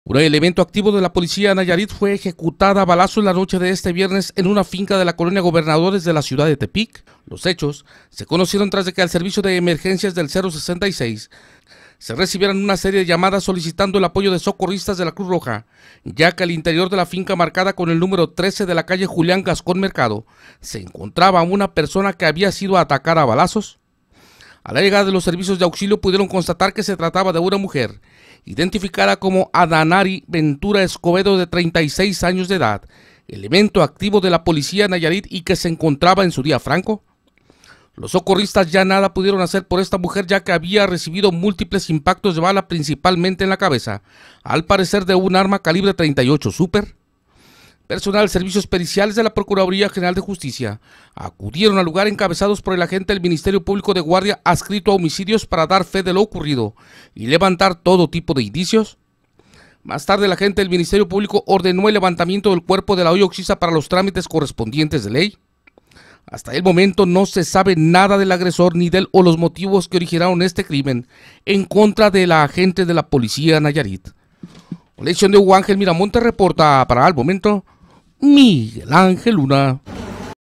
EJECUTAN A ELEMENTO DE LA POLICÍA NAYARIT VIDEO
POR RESPETO A LOS FAMILIARES DE LA PERSONA FALLECIDA, NUESTRO COMPAÑERO MIGUEL ÁNGEL LUNA, REALIZÓ TOMAS DISCRETAS Y SIN EL AFÁN DE LUCRAR CON EL AMARILLISMO NI OFENDER A NUESTROS QUERIDOS SEGUIDORES...